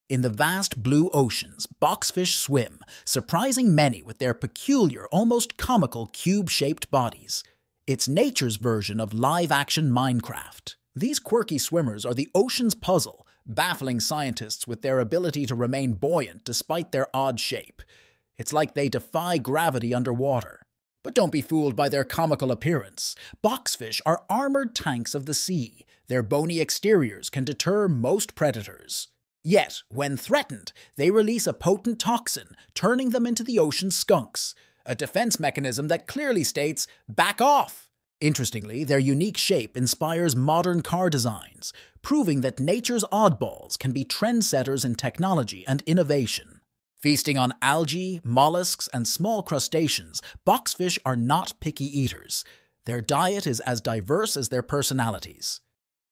What makes Boxfish special?
Dive into the fascinating world of Boxfish with this short video! Explore their unique box-like appearance, vibrant colors, and distinctive swimming style. Witness the Boxfish's incredible adaptability, from its armored exoskeleton to its slow, deliberate movements. Discover how Boxfish play a crucial role in marine ecosystems, showcasing their importance in maintaining underwater biodiversity. Join us for an underwater adventure as we unravel the mysteries of the captivating Boxfish!